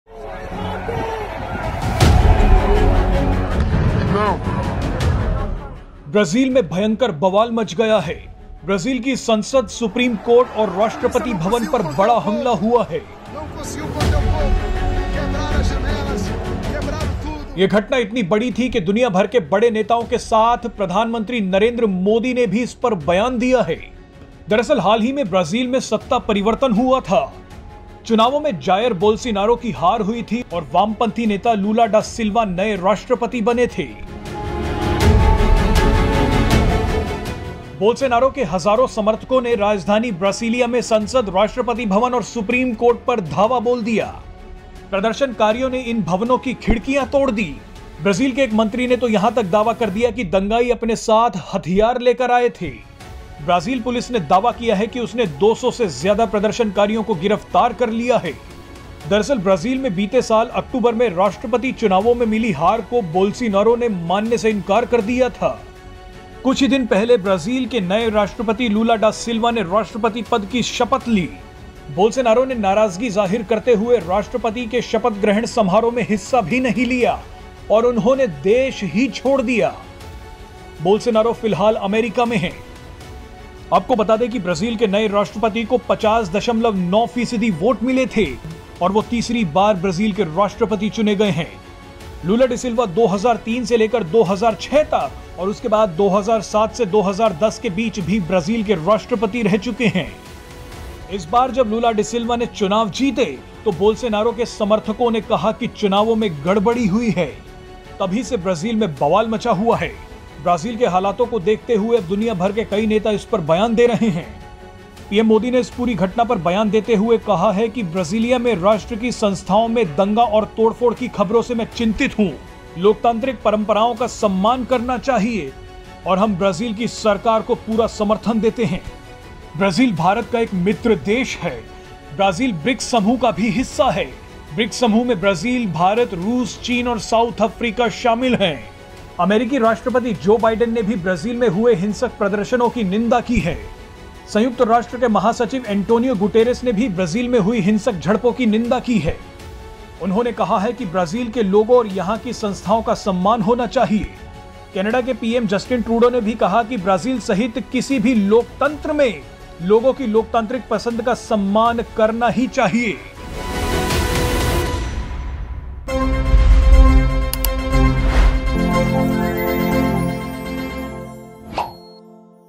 भारत के मित्र देश में हुआ बड़ा हमला, America भी हिल गया | PM Modi on Brazil Riots | Bolsonaro News
PM Modi expressed his concern over the riots in Brazil and said that democratic traditions "must be respected by everyone". "Deeply concerned about the news of rioting and vandalism against the State institutions in Brasilia. Democratic traditions must be respected by everyone. We extend our full support to the Brazilian authorities," PM Modi tweeted.

ब्राजील में पूर्व राष्ट्रपति बोल्सोनारो के समर्थकों ने तांडव मचाया, पुलिस पर पथराव किया, तोड़फोड़, आगजनी भी की.  बिगड़ते हालात को देखते हुए ब्राजील की राजधानी ब्रासीलिया में इमरजेंसी का ऐलान किया गया. प्रधानमंत्री नरेंद्र मोदी ने भी अपनी चिंता व्यक्त करते हुए कहा कि लोकतांत्रिक परंपराओं का सभी को सम्मान करना चाहिए.